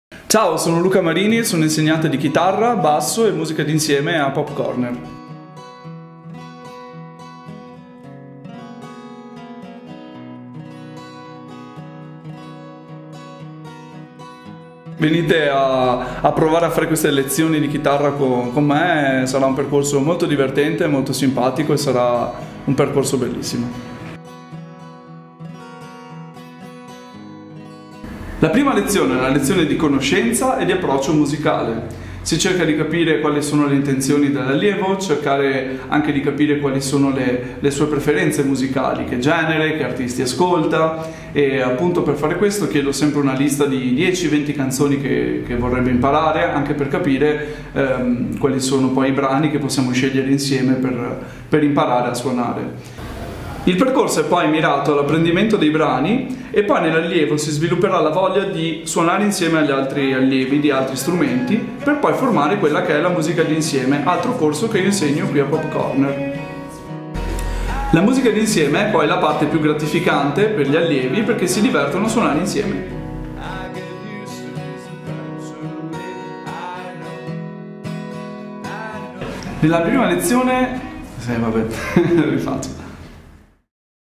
Luca Marini - Lezioni di Chitarra
Si apre la stagione 2020/2021 della nostra scuola di musica con la presentazione dei nostri docenti e dei nostri corsi.
Scopri la nostra offerta formativa in campo musicale e prenota la tua lezione gratuita per conoscerla di persona o tramite la piattaforma online.